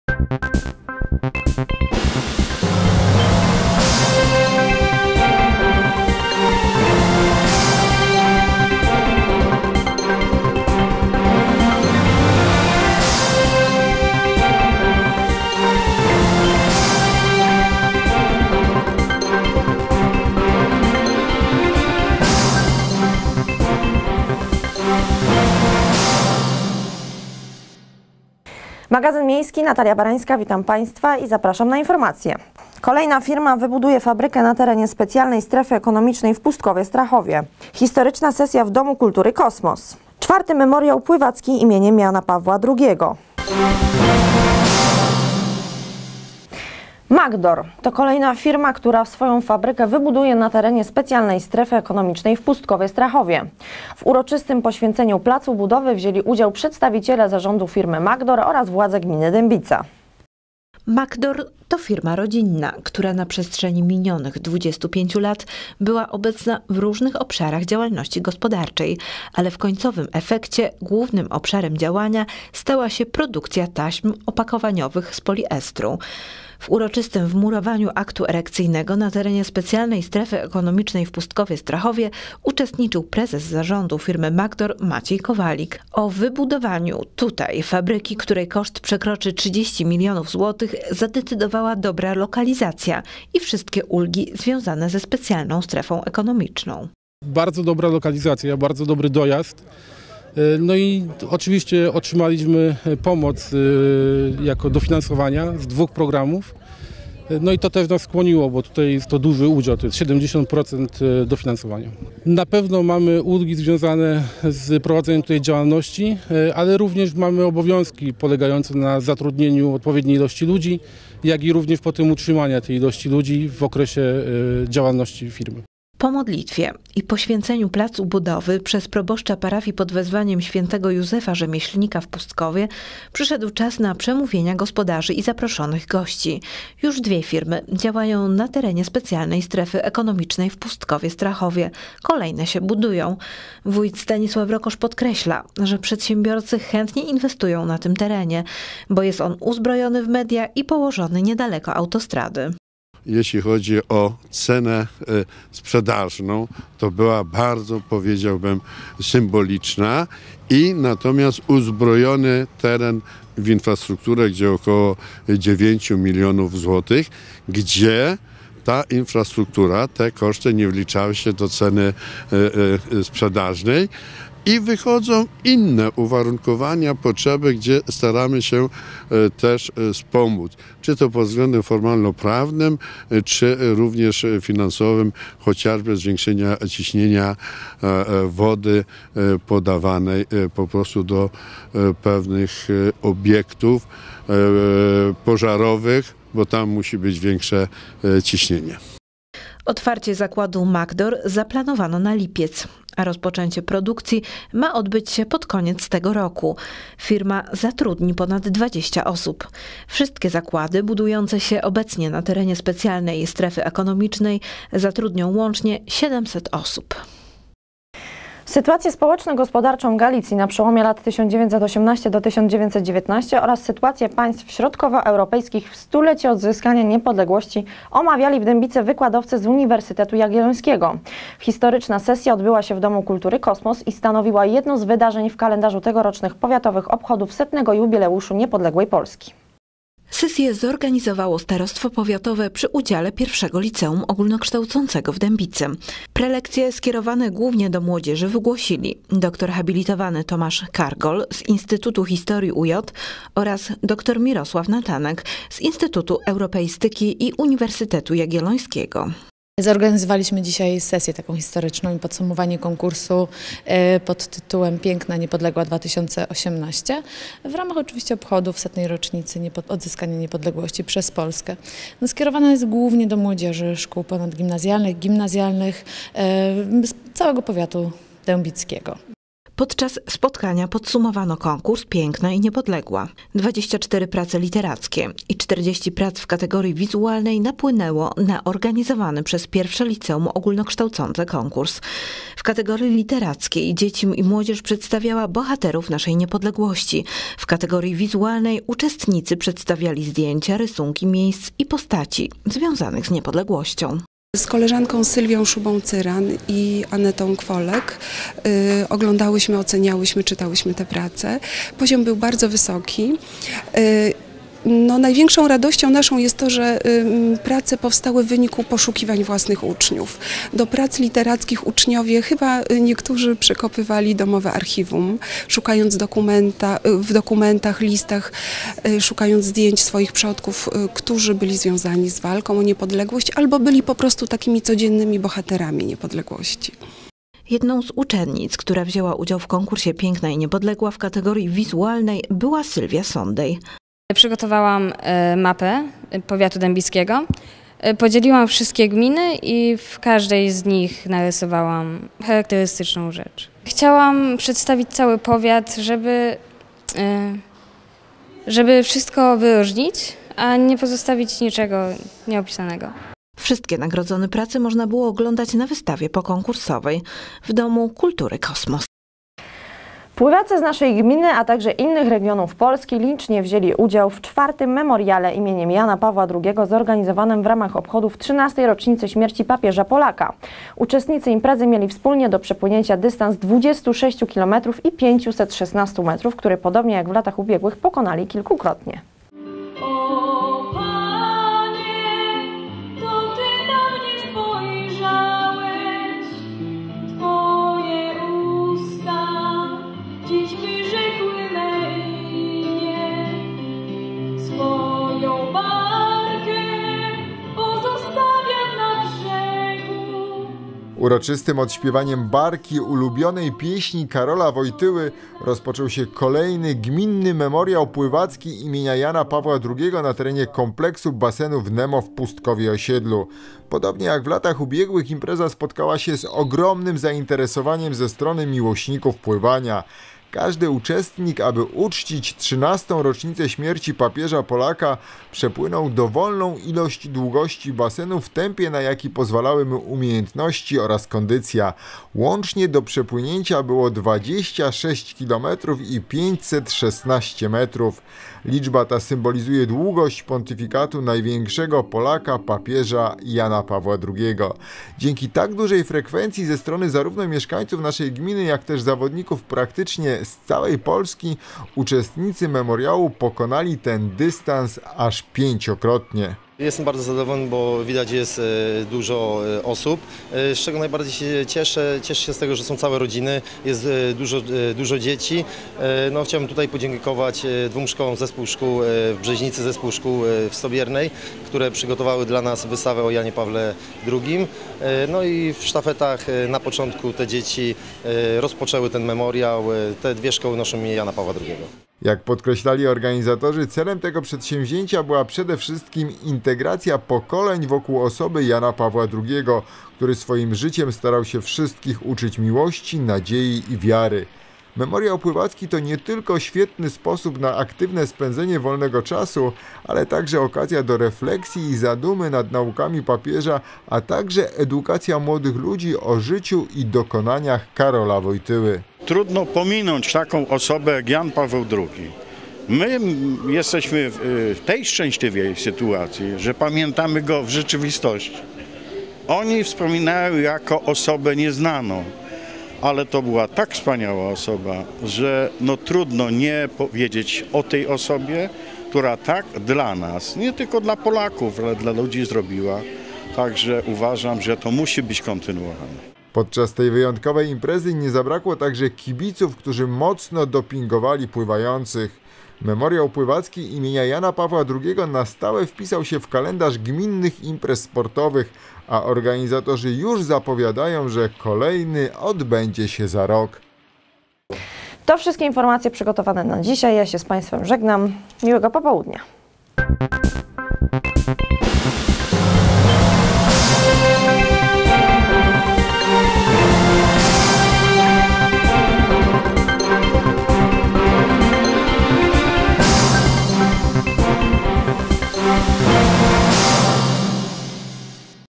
Magazyn Miejski Dębica 11.04.2018
1. Kolejna firma wybuduje fabrykę na terenie Specjalnej Strefy Ekonomicznej w Pustkowie – Strachowie
2. Historyczna sesja w Domu Kultury "Kosmos"
3. IV Memoriał Pływacki imienia Jana Pawła II